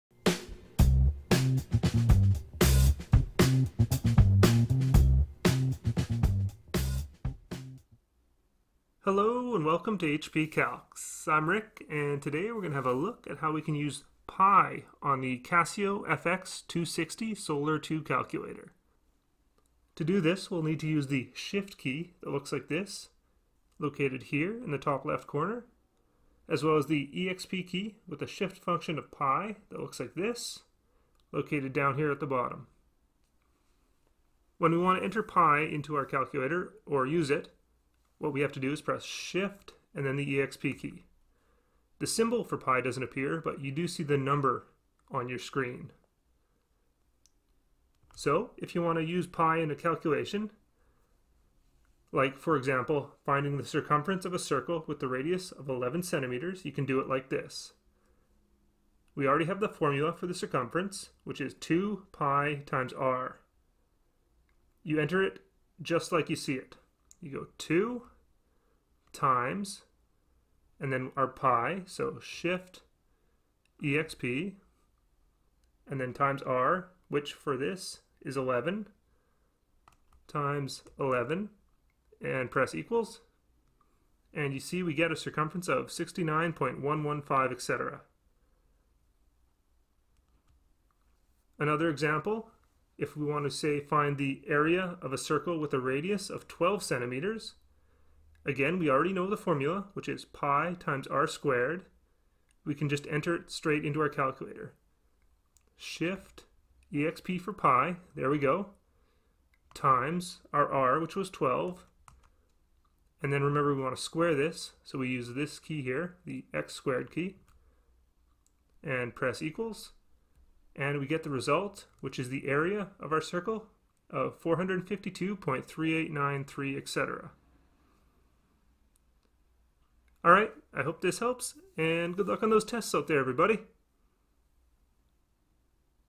How to use Pi on the Casio fx-260 calculator
Don't have this calculator?

A quick tutorial on how to use the Pi (π) function on the Casio fx-260 Scientific Calculator. Examples include finding the Pi key and using it to calculate the Circumference and Area of a circle.

Contents:
0:00 Intro
0:19 Buttons used
0:34 Entering Pi on the Calculator
0:47 Using Pi (Circumference of a circle)
1:30 Using Pi (Area of a circle)
2:09 Outro